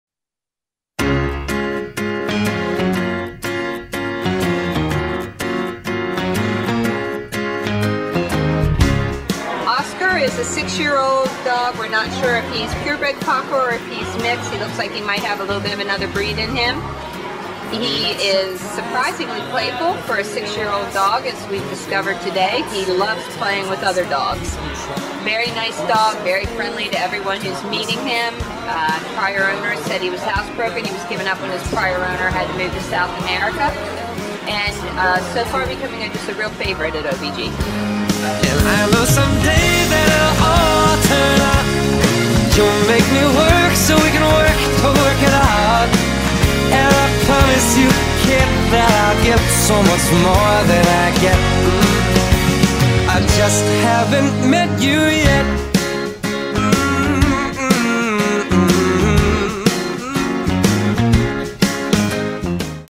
Oscar from OBG Cocker Spaniel REscue - ADOPTED
ADOPTED!
Male
Adult
Black
Playful & Loving

Hi! My name is Oscar and I'm a 6 year old fella. I was recently surrendered because my previous owners moved out the country and I wasn't allowed to move with them. I was very scared and nervous when I arrived at the animal shelter but fortunately the staff knew I was special and saw beyond my fears so they gave me loads of extra attention. They also contacted OBG because they knew I would be much happier in a foster rather than a crate. I'm glad they recognized a sweet and deserving boy when they met one!
I'm very fun to have around. I LOVE toys and enjoy playtime with other doggies. I ride nicely in the car and enjoy sitting on laps. I have an abundance of love to share and I'm hoping you're looking for unconditional love just like me! If so, drop me a line so we can get to know each other!

Kisses,
Oscar